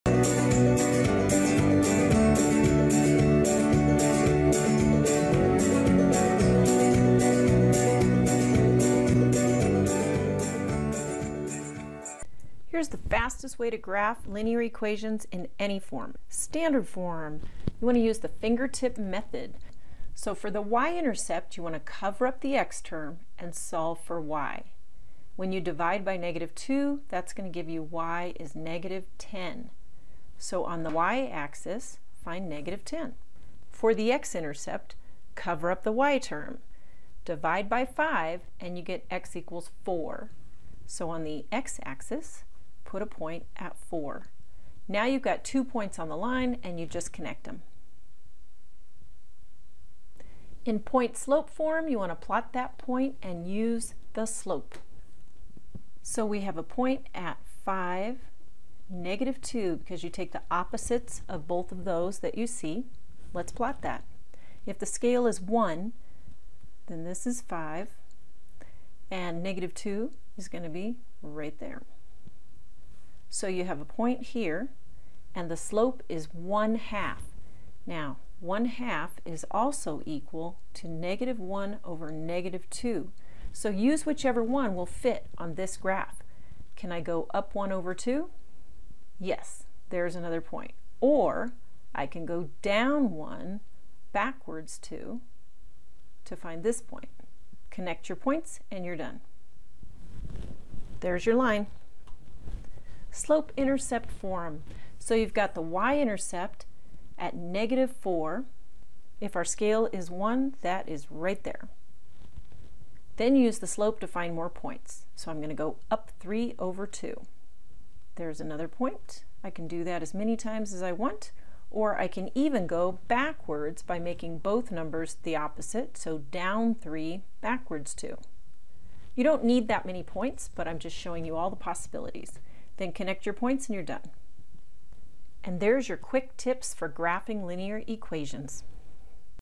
Quick Tips for Graphing Linear Equations
The fastest way to graph a line from each form: standard, slope intercept, and point-slope.